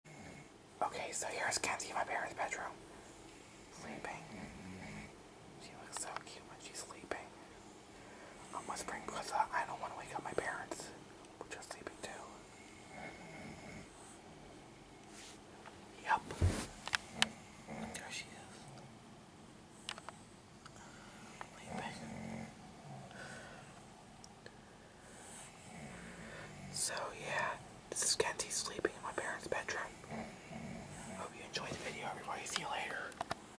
Kenzie Sleeping in My Parents' Bedroom - 3/2/19
Here is a video of my Cairn terrier dog Kenzie sleeping in my parents' bedroom on Saturday, March 2, 2019 (my 23rd birthday). Enjoy!